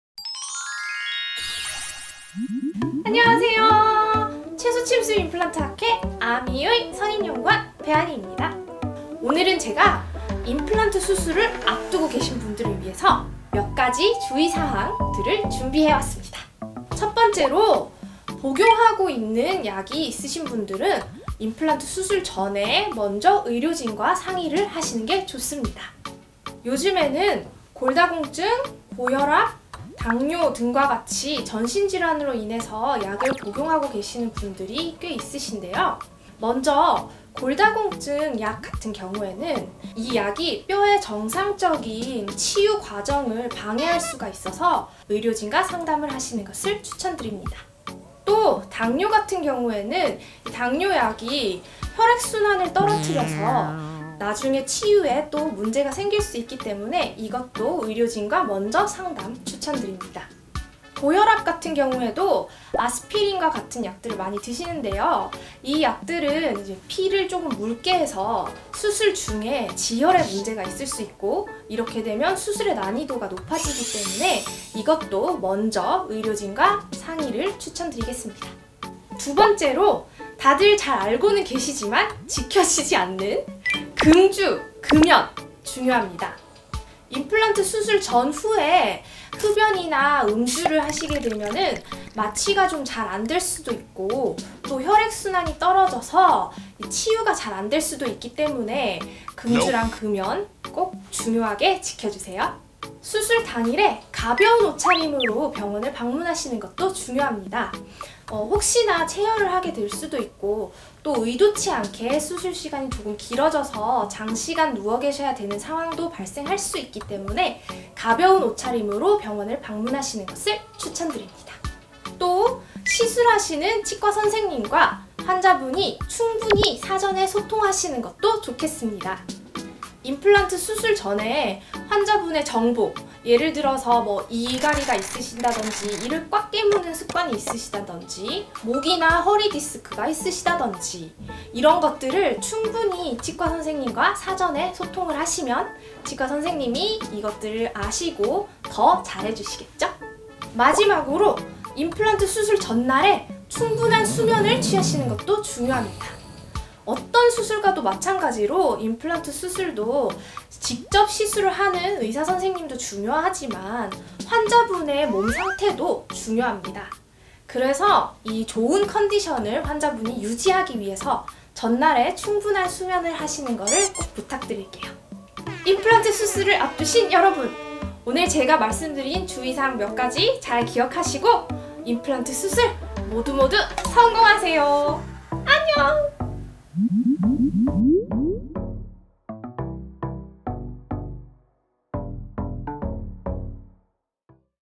배틀 임플란트 Episode4 - 임플란트 수술 전 주의사항 🚫🚨
배하늬 연구원의 틀니말고 임플란트!
[배틀 임플란트 4화]

📢 임플란트 수술을 앞두신 여러분~
배하늬 연구원님이 알려주는
임플란트 수술 전 주의사항 알아보고 임플란트 수술 성공하세요! 👍

궁금하신 점이 있으시다면 댓글도 많이 남겨주세요 🤗

AMII 세미나, 연수회 교육 일정 확인 및 신청